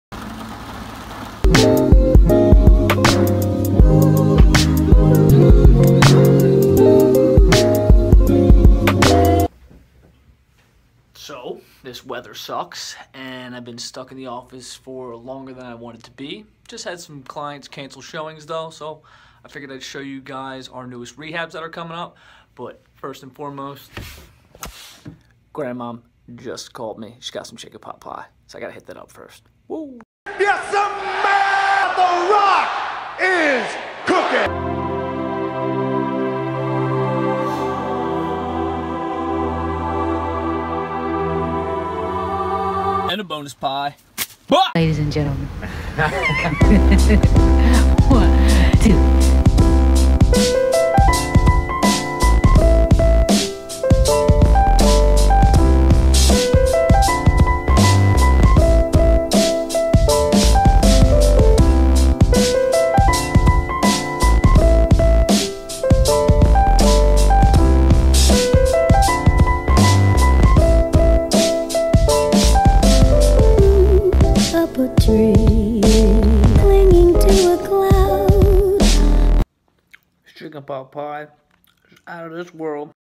House Rehabs & Grandmom's cooking
A look at some rehab projects that are coming up.
Bonus! Some of Grandma's cooking!